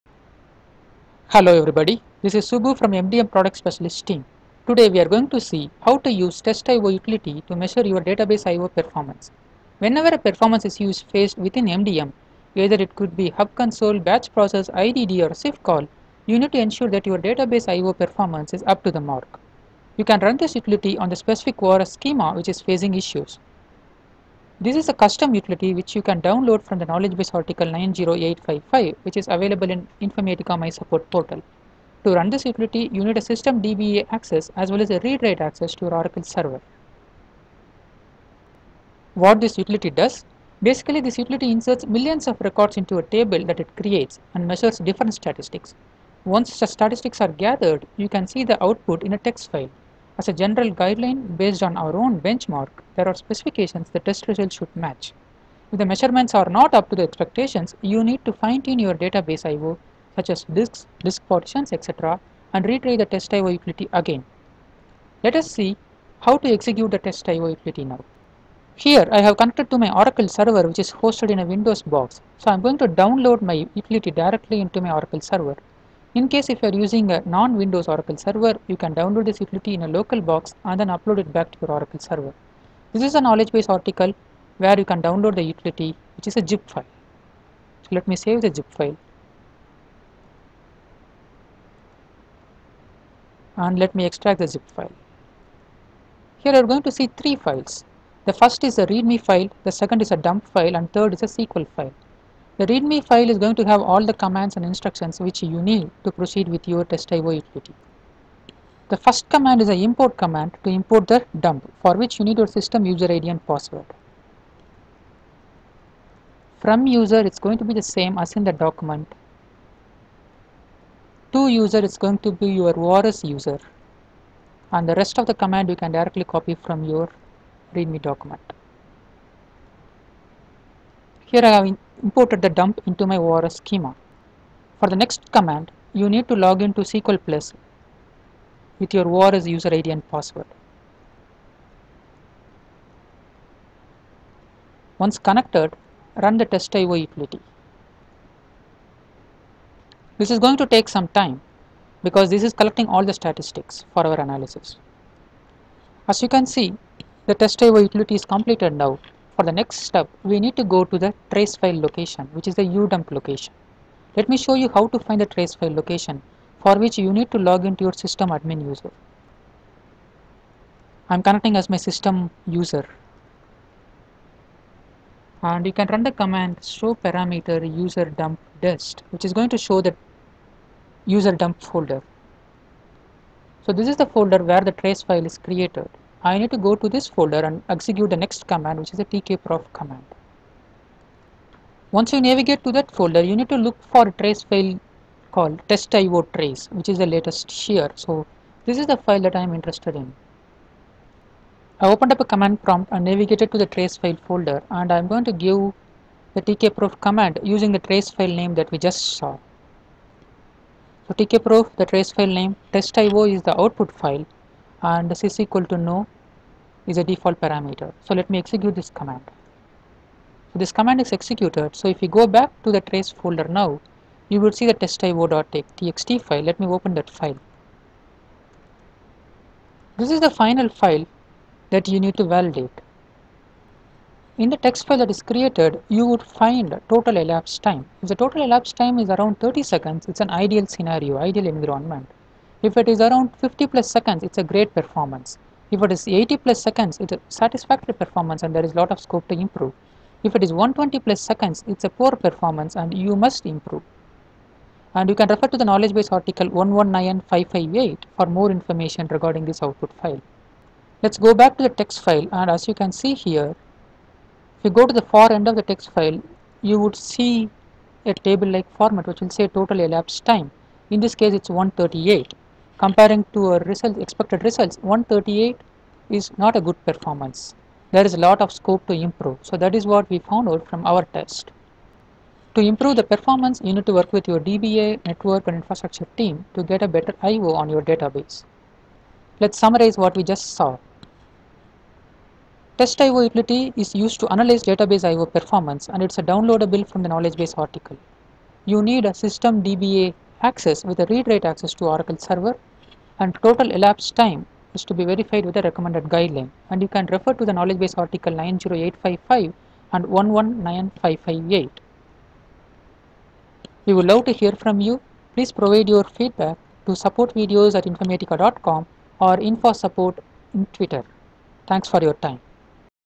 How to run Test IO utility
This video demonstrates how to run the Test IO tool.